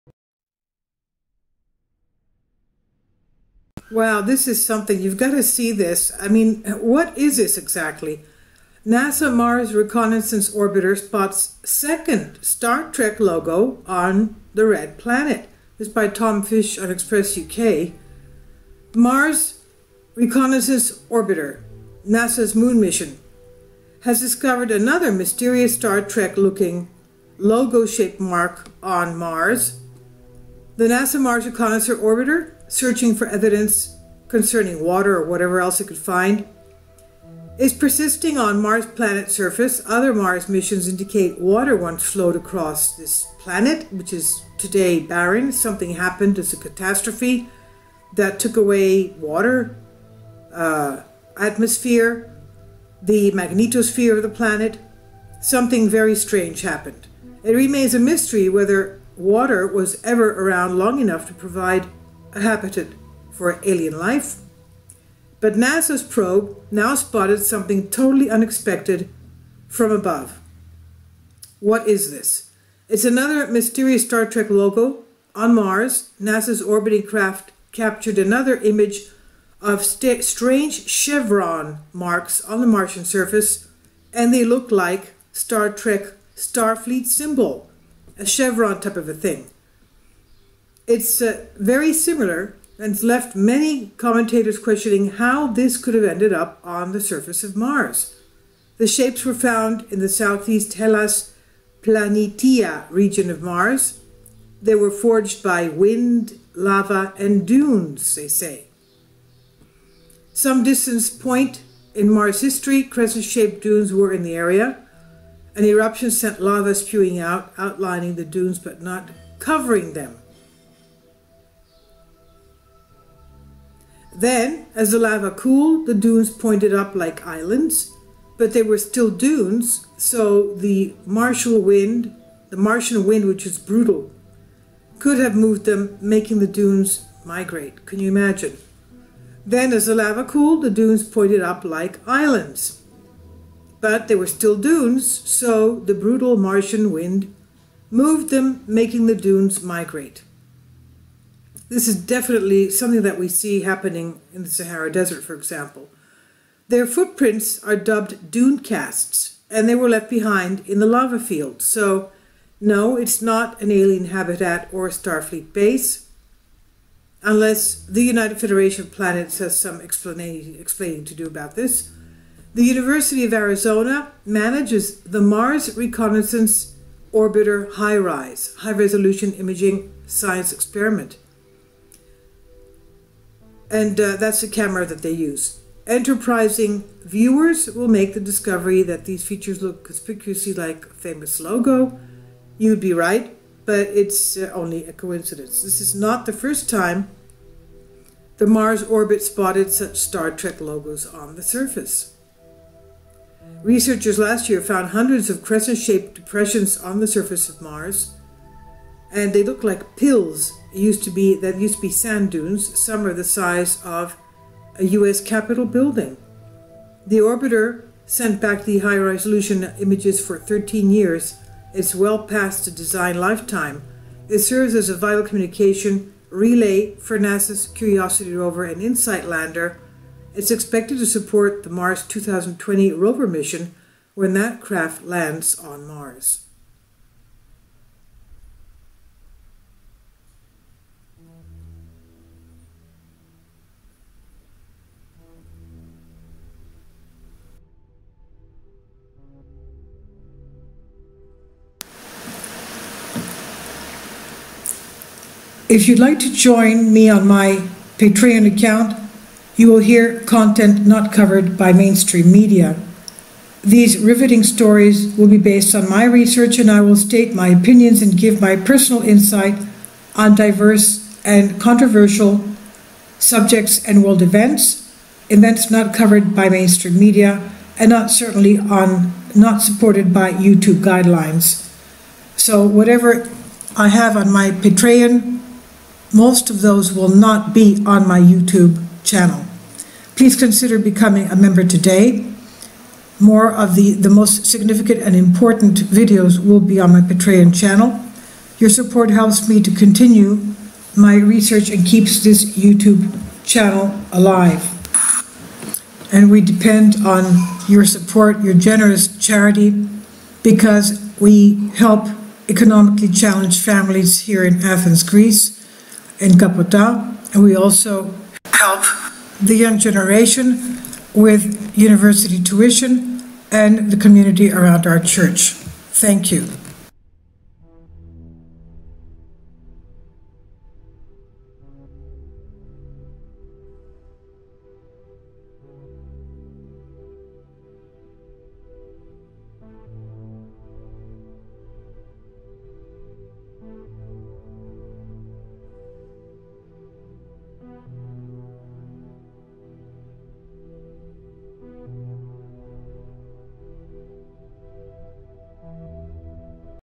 NASA Mars Reconnaissance Orbiter spots SECOND Star Trek logo on Red Planet
2) Thumbnail image - mars star trek symbol nasa.jpg